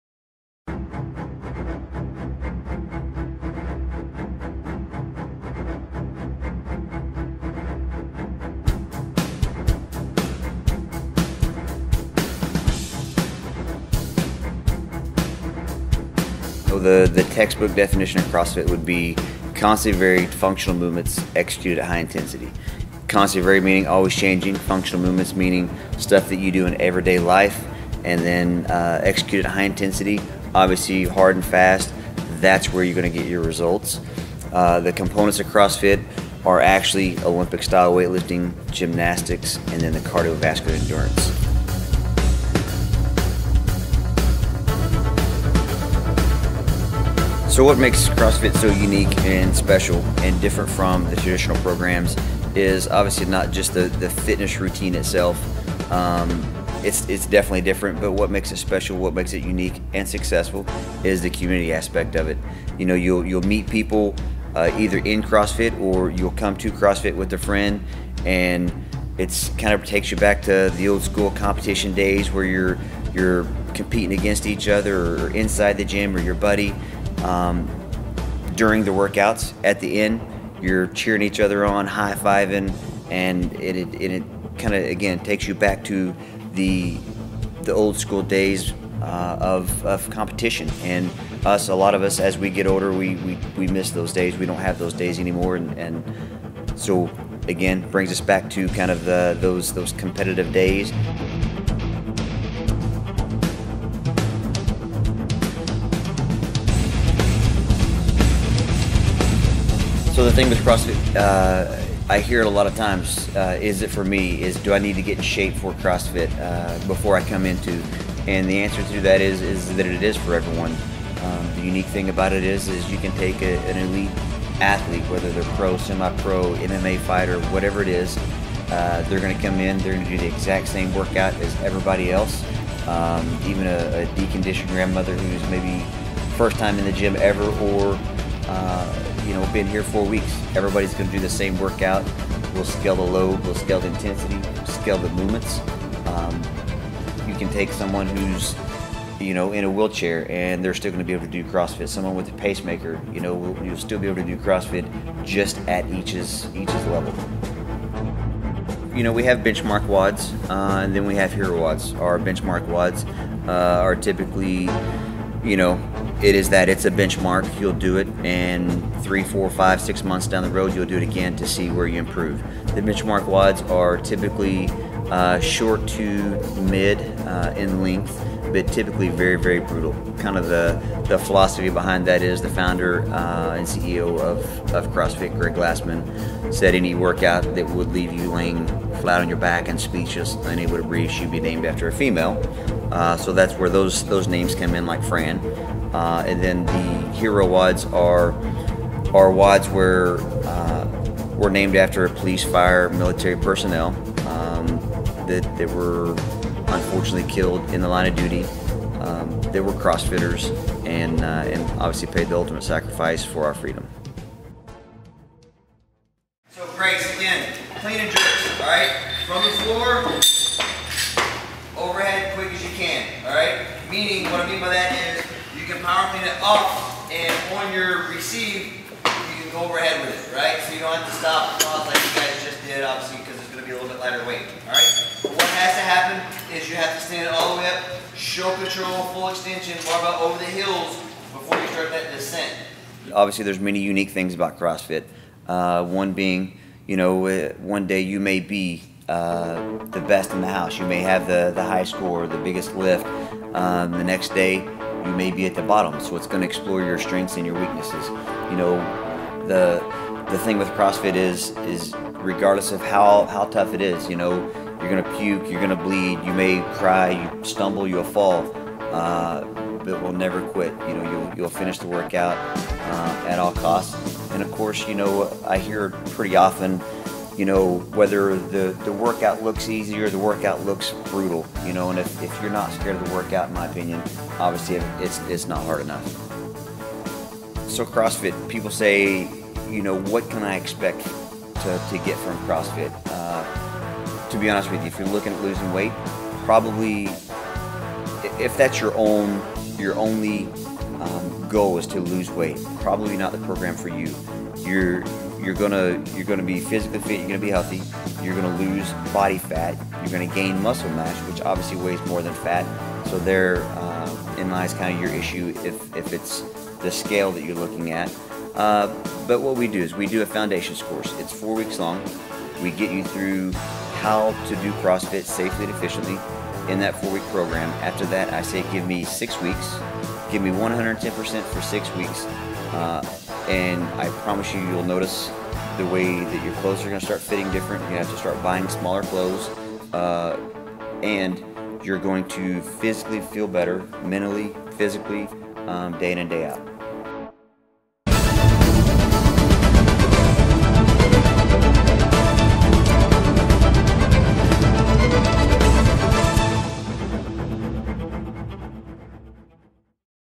Cross Fit NWA
Take a look deep inside the work out phenomenon, Cross Fit.  We explore one of the most successful boxes in the nation as we tour Northwest Arkansas with Lee Kelly.